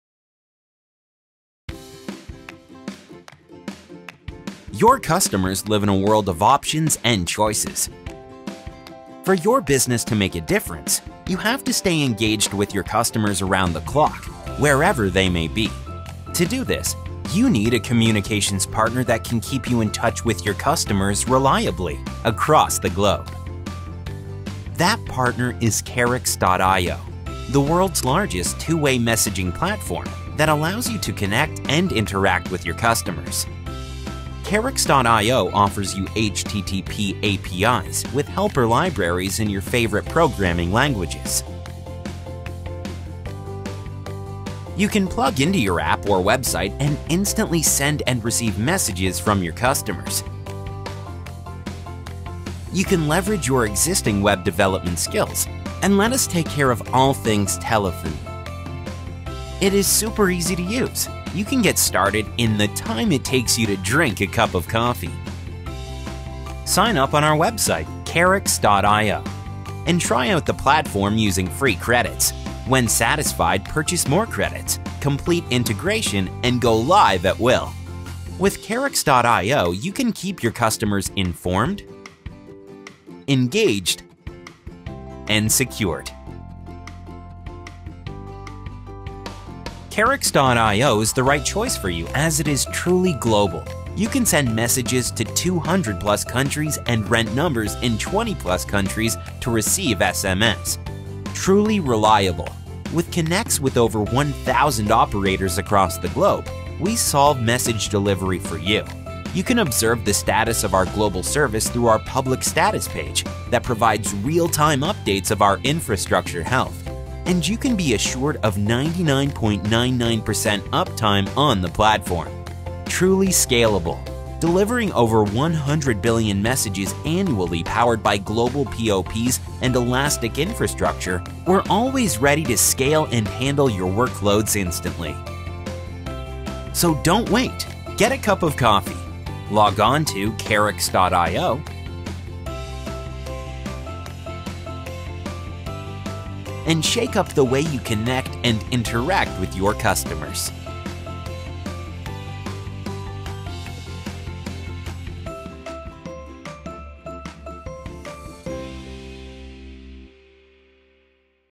Karix.io - Cloud Messaging APIs for Software Developers & Businesses
With karix.io, businesses and app developers with no skills in telecom, can build state-of-the-art applications that are capable of sending automated messages such as transaction notifications, billing information, renewal messages, feedback etc. to customers across 200 plus countries.